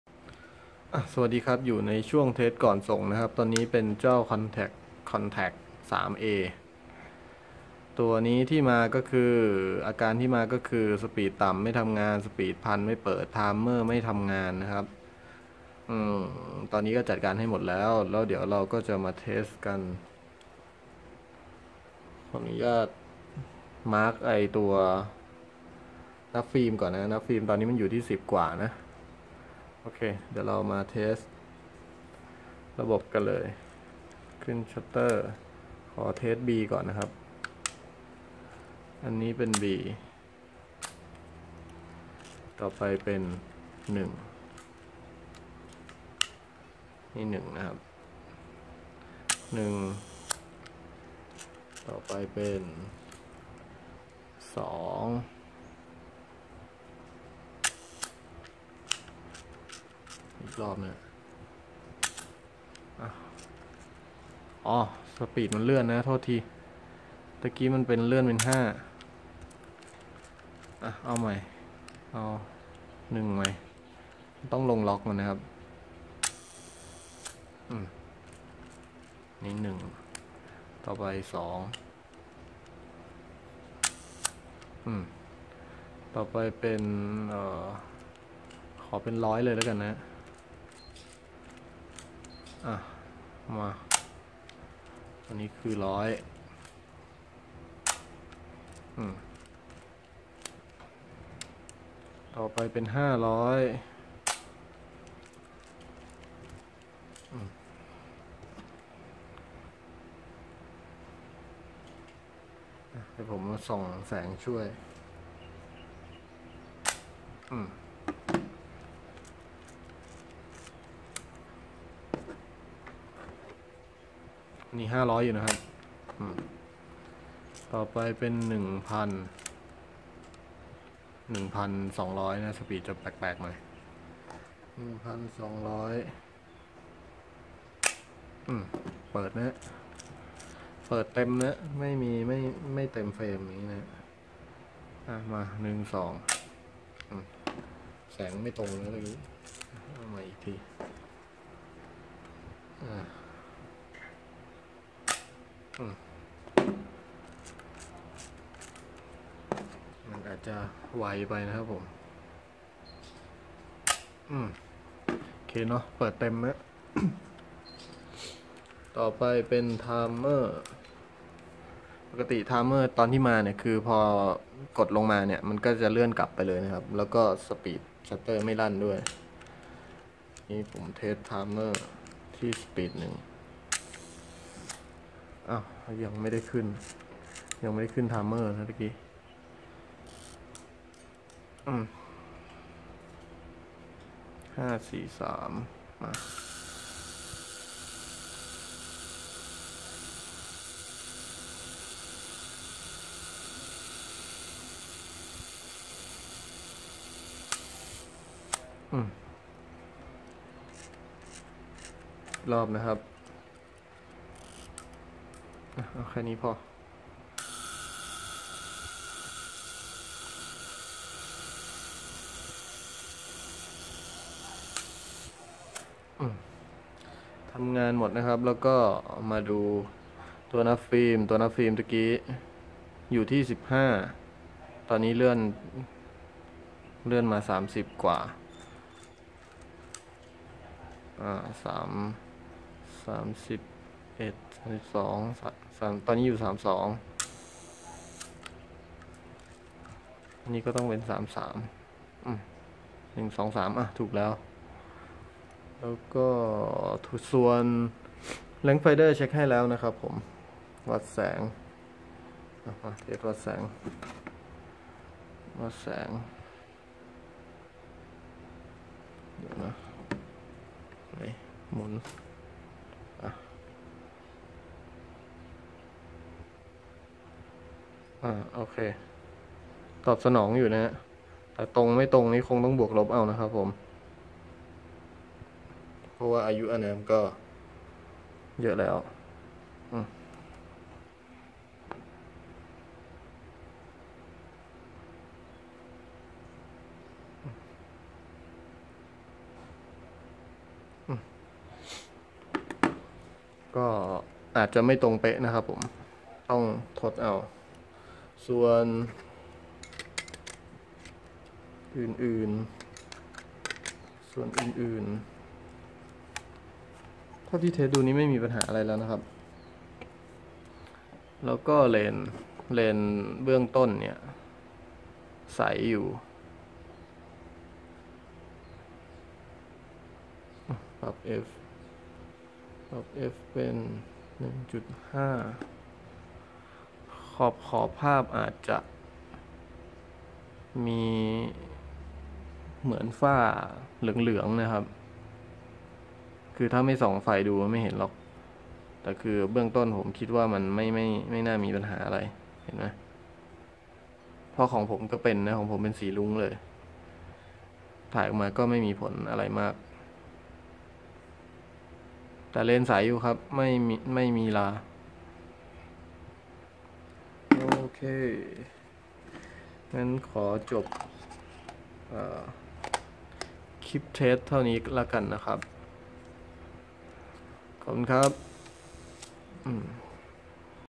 ดนัยซ่อมกล้อง : เทสก่อนส่ง Zeiss Contax IIIa (SNxxx405)
รักษาหายแล้วกลับบ้านได้😀~

ท่านใดจะส่งกล้องเข้ามาปรึกษาได้ที่ inbox เพจ Facebook
"ซ่อมกล้องฟิล์มbyดนัยค้าขายกล้อง"

ขอบคุณลูกค้าทุกท่านที่ไว้วางใจนะครับ😀😀😀